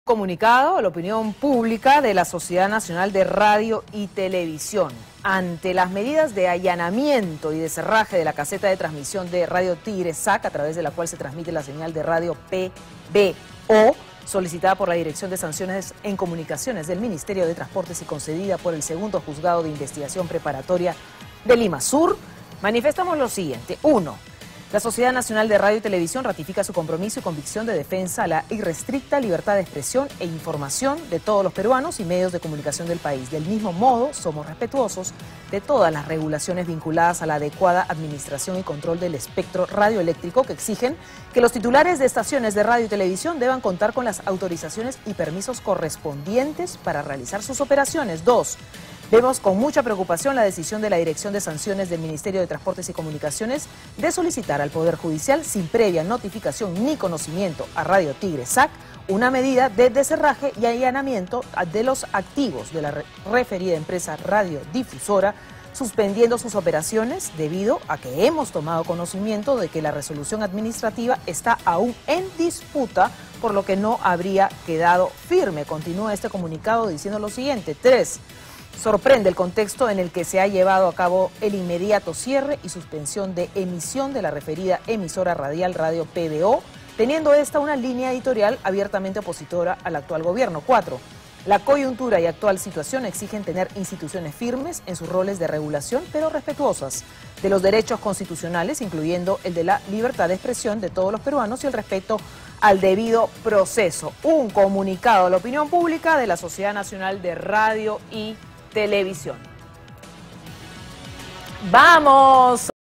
SNRTV ratificó su compromiso con la defensa de la libertad de expresión
Tras el allanamiento del Ministerio Público a la estación radial PBO 91.9 F.M (radio Tigre S.A.C) la Sociedad Nacional de Radio y Televisión (SRNT) se pronunció al respecto y ratificó su compromiso y convicción de defensa a la libertad de expresión e información.

"La SRTV ratifica su compromiso y convicción de defensa a la irrestricta libertad de expresión e información de todos los peruanos y medios de comunicación del país", se lee en el comunicado.

Además, el gremio, mostró su preocupación ante la decisión de la Dirección de Sanciones del Ministerio de Transportes y Comunicaciones de solicitar al Poder Judicial, sin previa notificación, el allanamiento y deserraje de una caseta de transmisión de la antes mencionada estación radial en el Morro Solar

En esa línea, el SNRTV consideró como sorprendente "el contexto" como se llevó a cabo el cierre y suspensión de la emisora referida.

RADIO PBO FUE ALLANA POR EL MTC

Cabe recordar que en horas de la mañana, funcionarios del Ministerio de Transportes y Comunicaciones, un  fiscal y personal policial allanaron las instalaciones de la estación radial PBO, de donde incautaron equipos de telecomunicaciones por no contar con una licencia de funcionamiento emitida por el MTC.